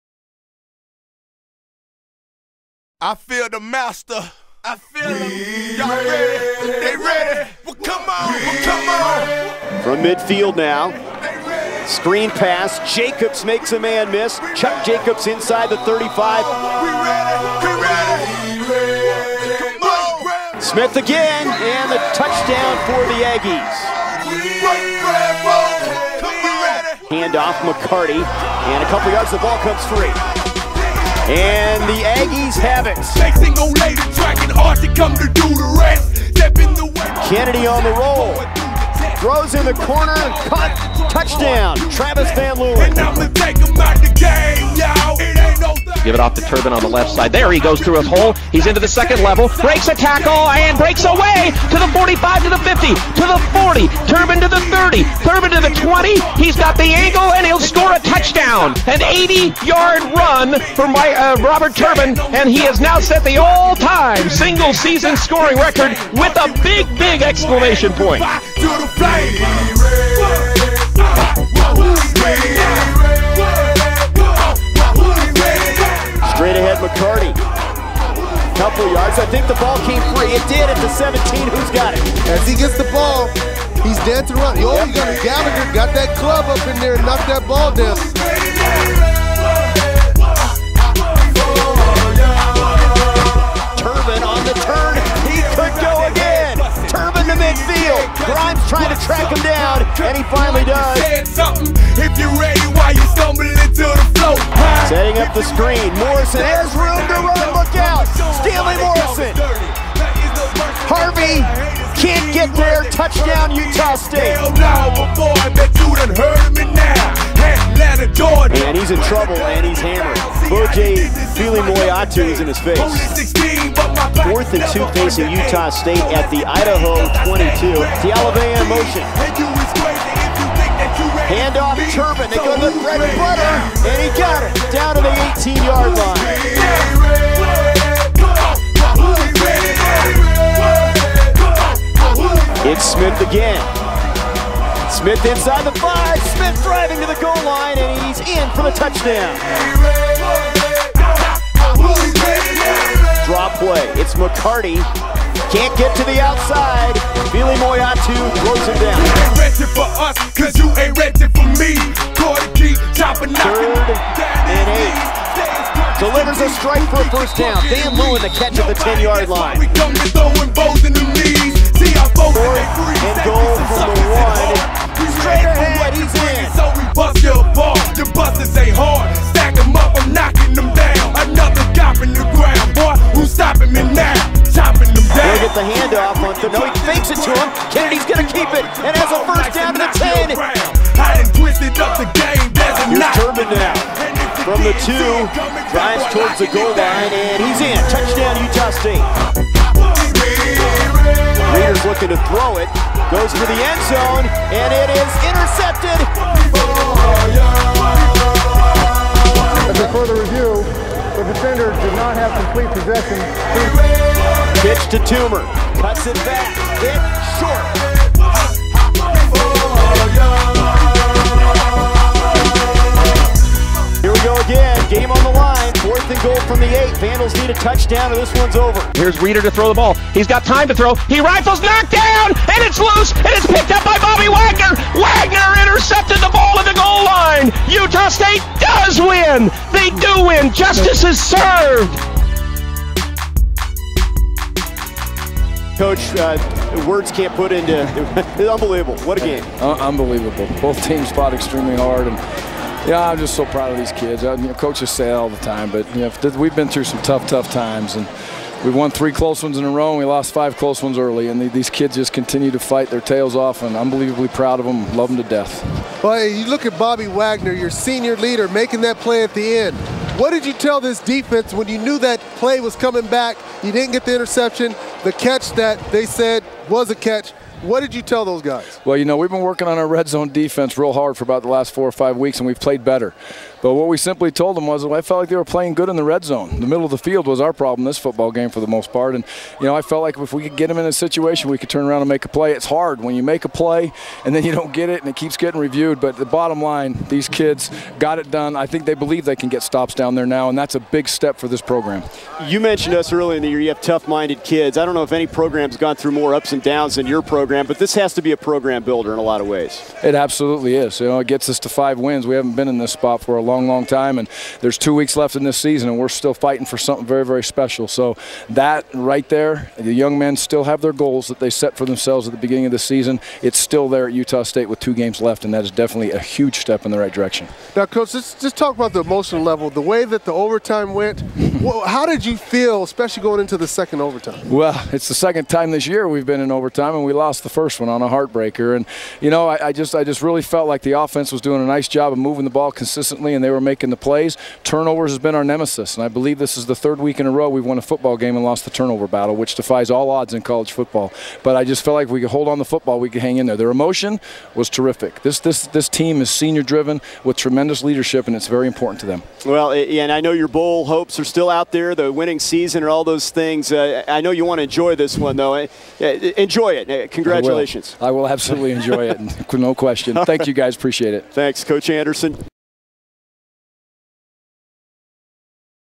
UtahStateFootball: Aggies Win 49-42 Thriller in Dbl OT
Utah State Aggies vs. Idaho Vandals; November 19, 2011
Produced by Zach Nyborg